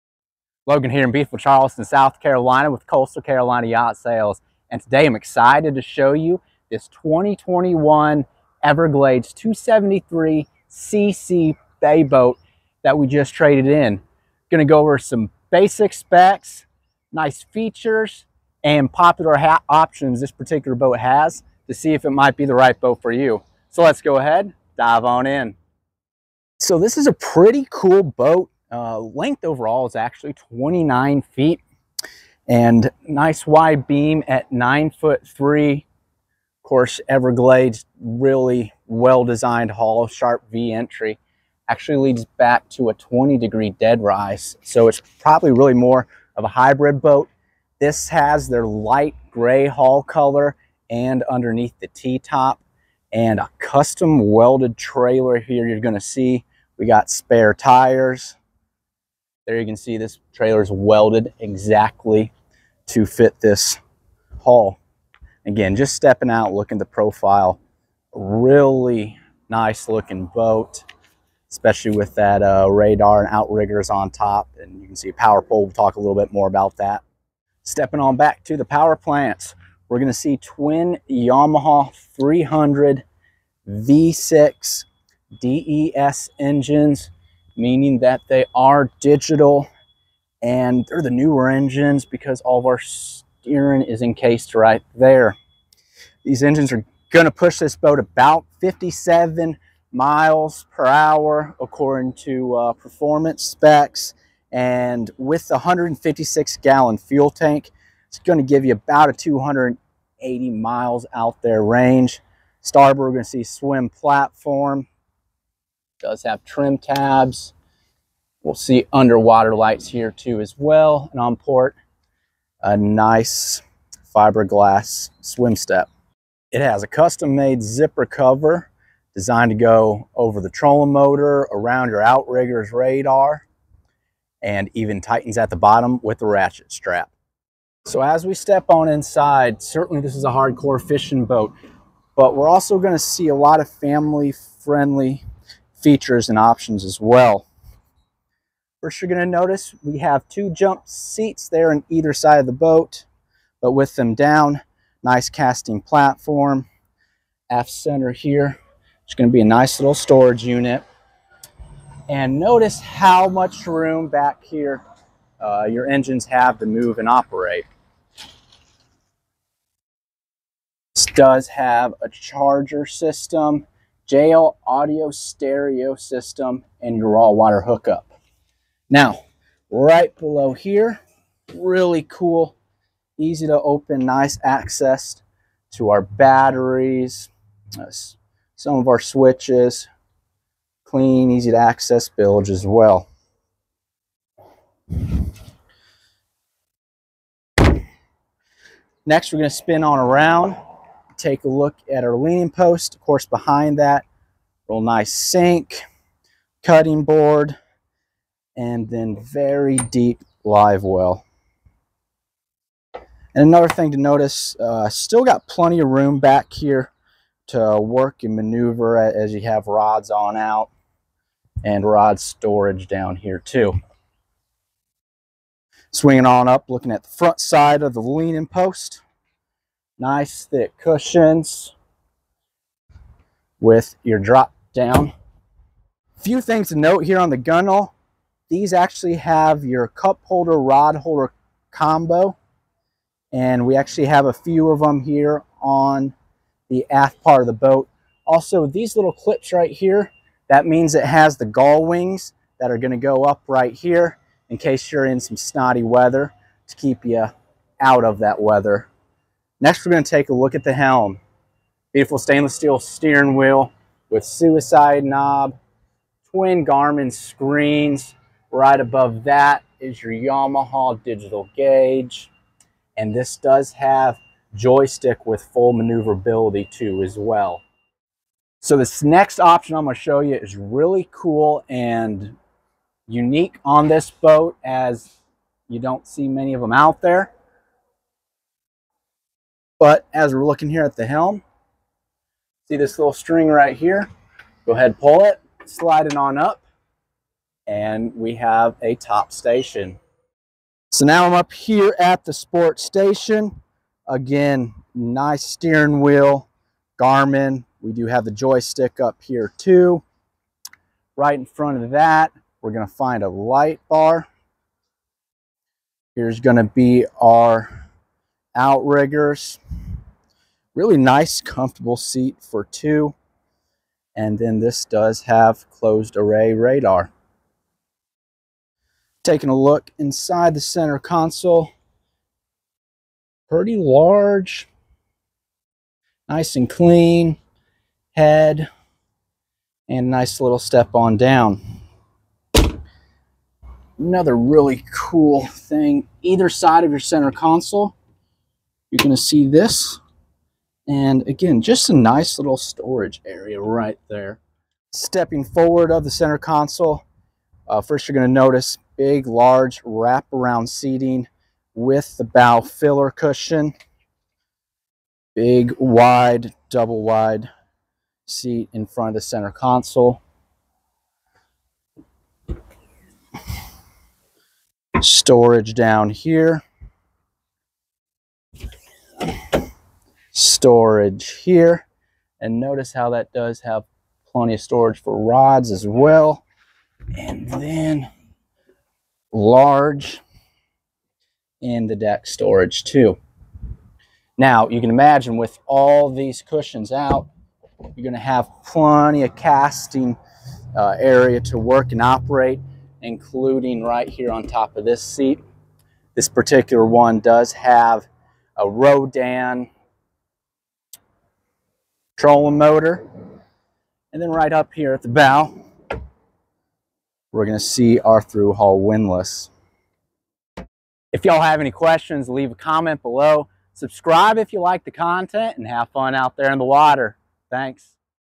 Everglades 273 CC Bay Boat Walkthrough - Amazing Fishing Boat with Family Friendly Features
The Everglades 273 CC Bay boat is a hard core fishing boat with family friendly features. It has twin Yamaha 300s with full maneuverability helm master joystick and auto pilot. There are three 16" Garmin 8616 XSV screens; two at the hem and one at the sport station. Garmin GMR 18HD dome ratar and Garmin VHF included. It has Yamaha CL7 gauge. Fusion stereo with subwoofer included. Rhodan trolling motor and PowerPole are also on the boat. It comes with a custom Magic Tilt welded aluminum trailer. It has a custom full boat cover.